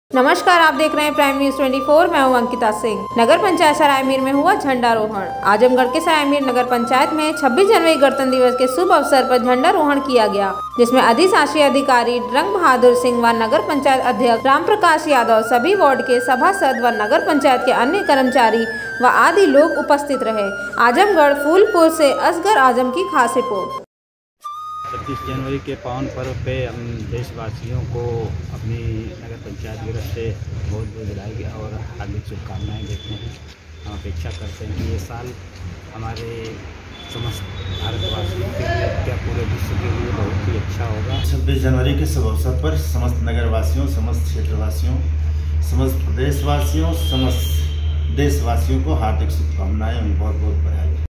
आजमगढ़ के सरायमीर नगर पंचायत मैं 26 जनवरी गणतंत्र दिवस के शुभ अवसर पर झंडारोहण किया गया
नगर पंचायत सरायमीर में हुआ झंडा रोहण

आजमगढ़ के सरायमीर नगर पंचायत मैं 26जनवरीगणतंत्रदिवस के शुभ अवसर पर झंडारोहण किया गया जिसमें अधिशासी अधिकारी रंग बहादुर सिंह वह नगर पंचायत अध्यक्ष रामप्रकाश यादव सभी वार्ड के सभासद वा नगर पंचायत के अन्य कर्मचारी वा आदि लोग उपस्थित रहे

आजमगढ़ फूलपुर से असगर आज़म की खास रिपोर्ट